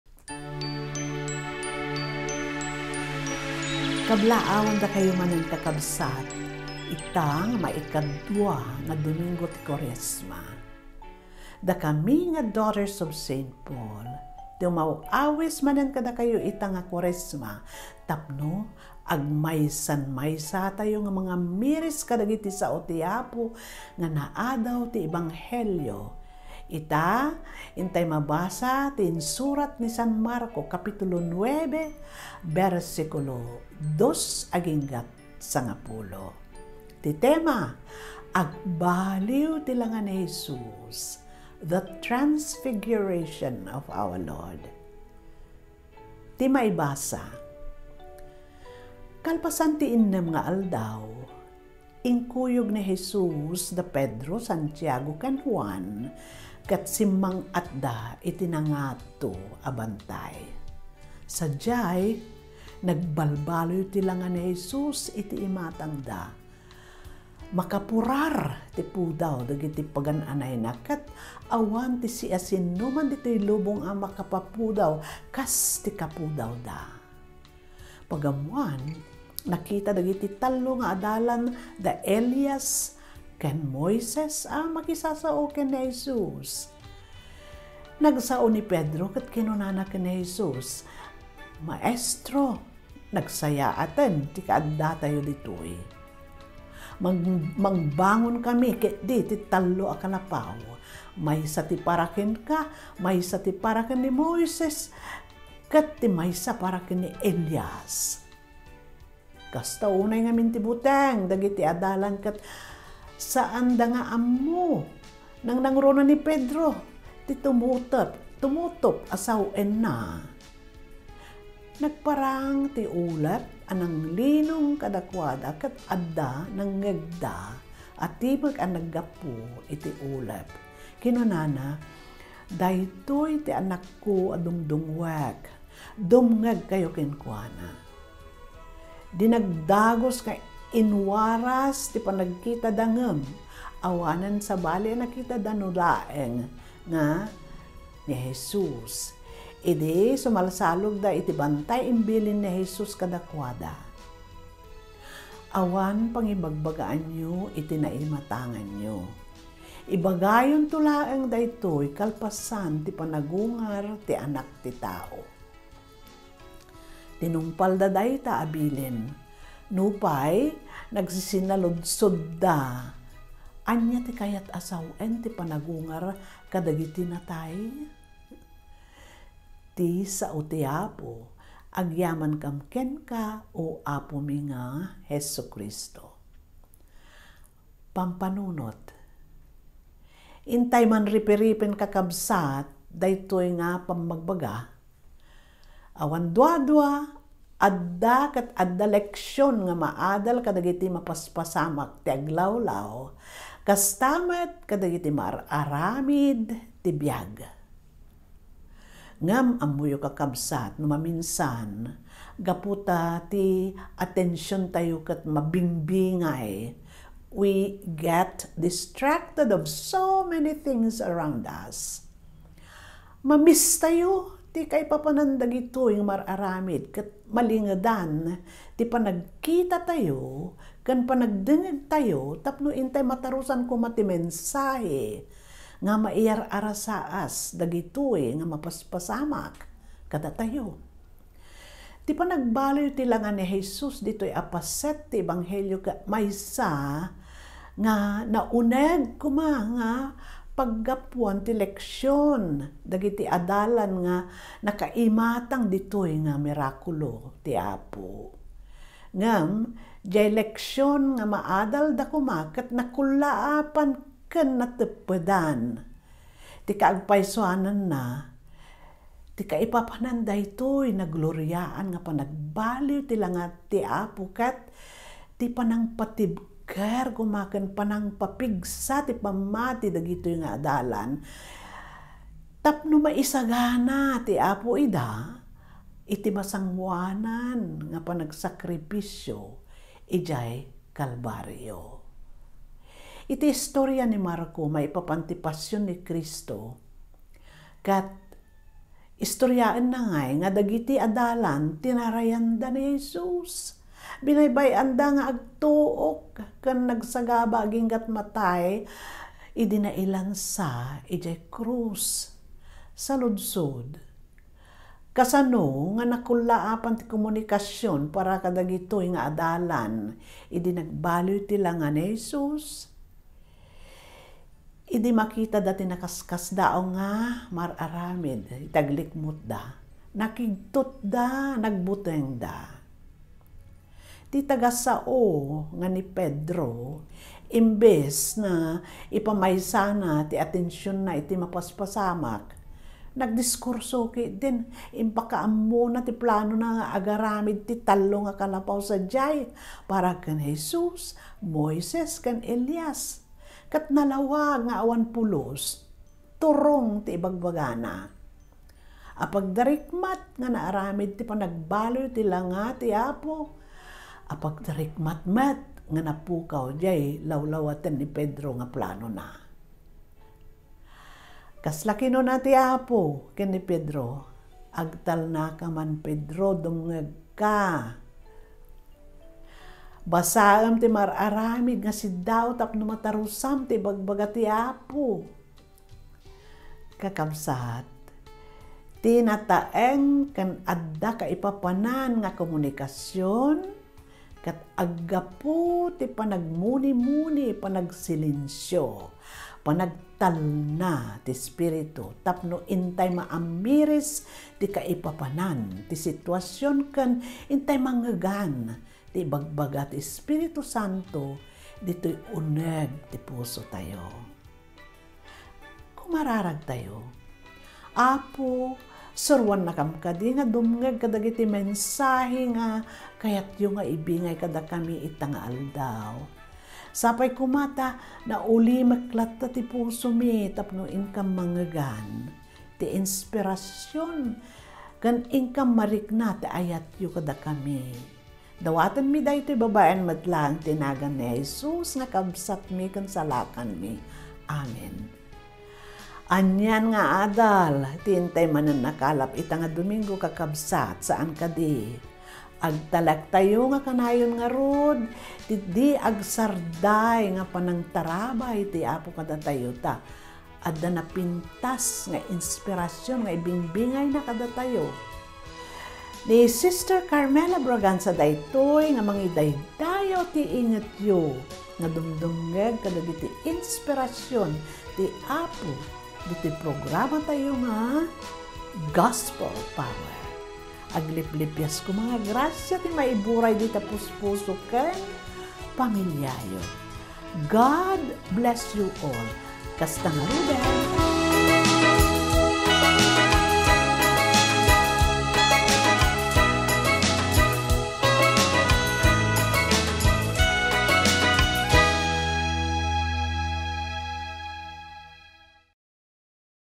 Gospel Power (Ilocano) - February 25, 2024 | 2nd Sunday of Lent
with Sr. Carmela Braganza, fsp | Daughters of St Paul, Tuguegarao City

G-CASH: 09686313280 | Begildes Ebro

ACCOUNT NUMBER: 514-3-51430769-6
SWIFT CODE: MBTCPHMM